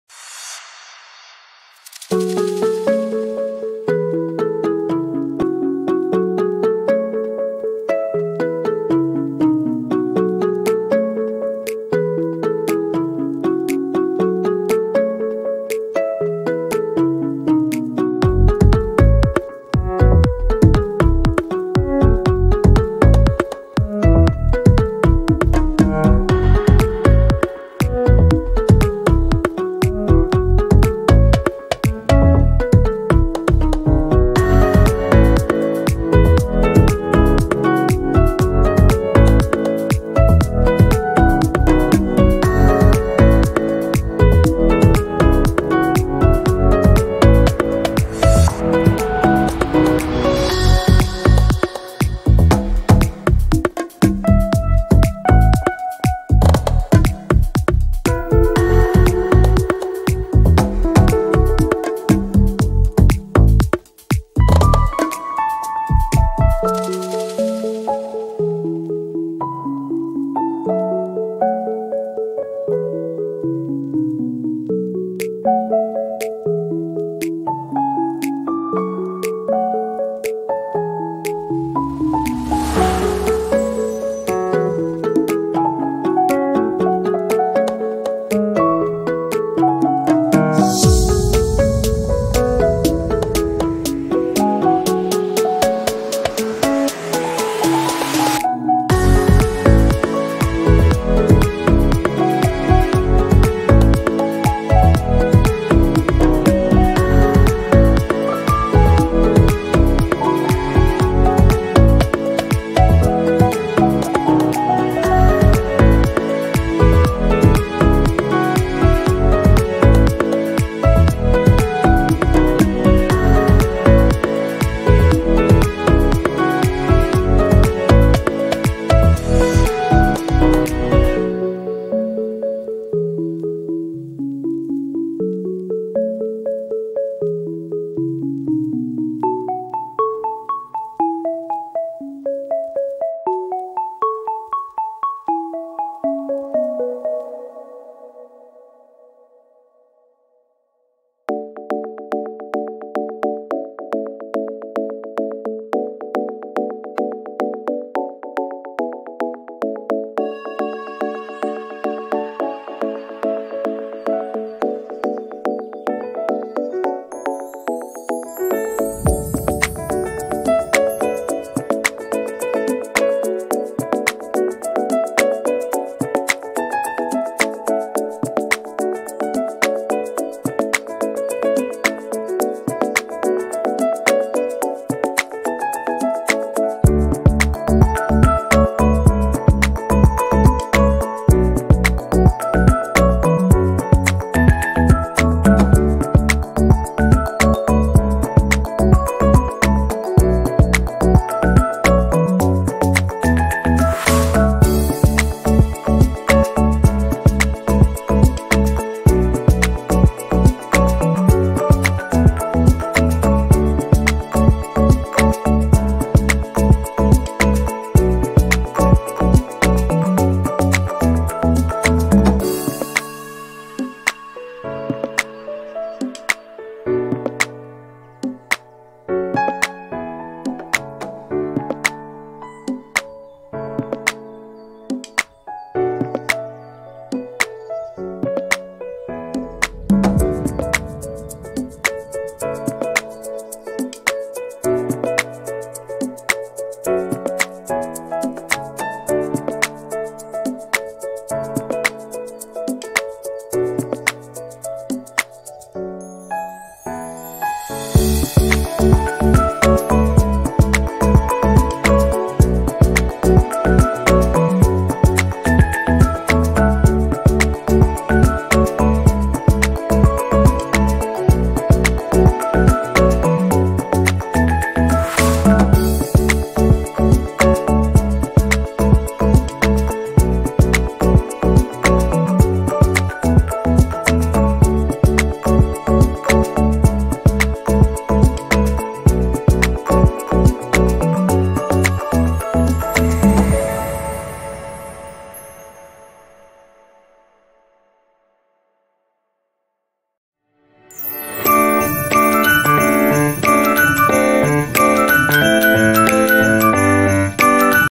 Nidal_drawing. Bhabhiji ghar par hai. Bhabhiji ka sketch. &TV. Shubhangi Atre. Shubhangi.
Bhabhi ji ghar par hai is a popular TV show broadcasting in &TV. Took inspiration for bhabhiji's facial expressions and I tried copy this in my her portrait.